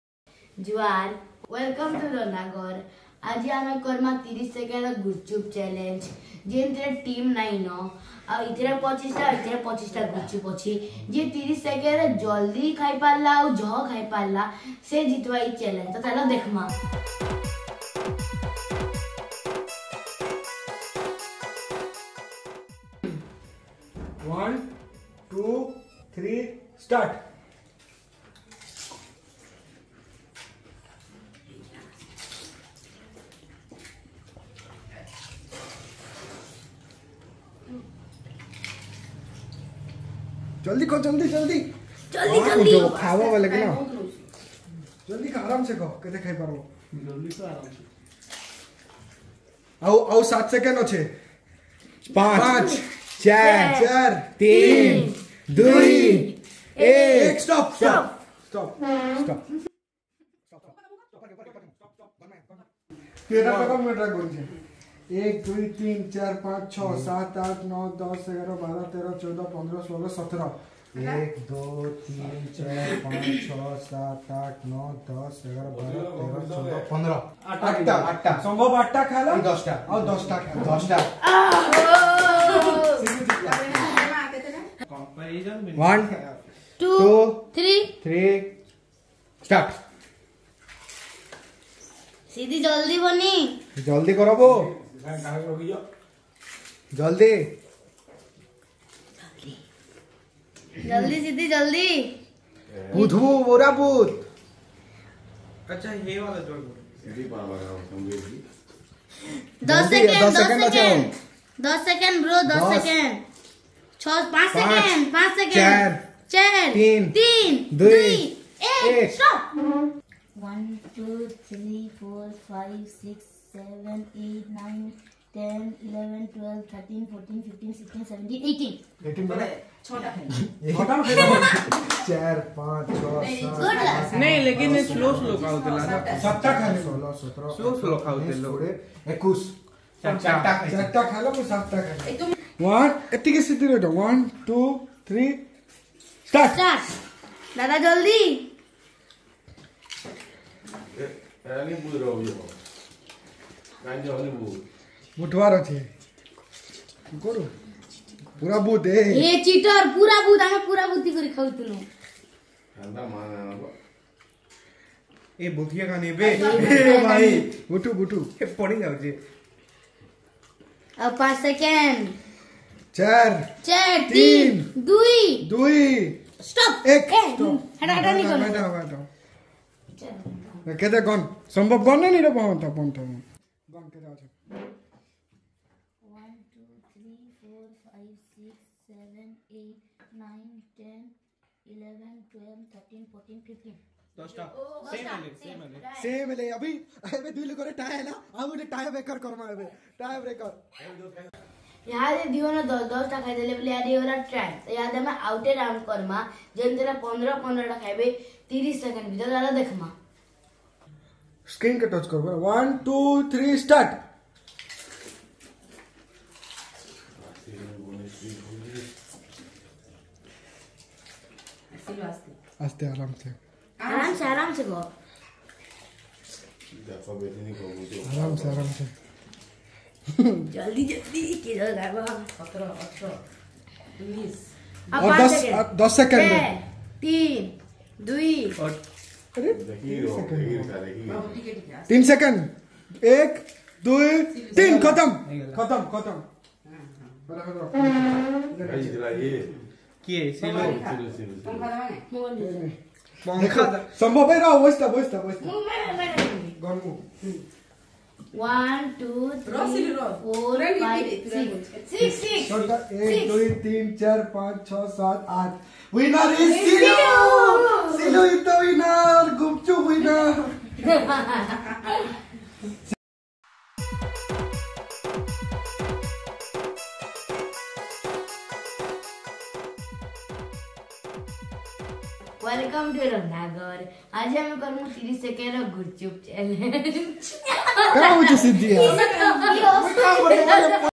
30 Sec Golgapa challenge | Panipuri challenge | food challenge | fun & masti | Randha Ghar 0.32
The Golgapa challenge will decide the winner of Randha Ghar this Sunday. After many discussions, my children decided to take Golgapa as the food for the challenge.
This time there is no such team. All will play individually. So let's see who wins the game?s
The rule: How many Panipuri can you eat in 30 sec?